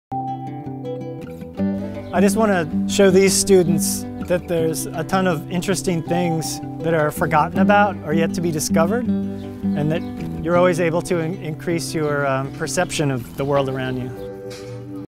Open Source in 15 Seconds: MOMO
Here's #8 of 14 behind-the-scenes videos about the artists of Open Source and how they're exploring the diverse communities and neighborhoods of Philadelphia, thanks to their work with Mural Arts. For Open Source, MOMO is working with our Art Education students and teaching them how Practical Geometry, classic techniques used by carpenters and craftspeople, can be used to create abstract art. He's also working with them on a mural at Frankford and Berks.

For more info on Open Source, including a list of project sponsors, visit opensource.muralarts.org.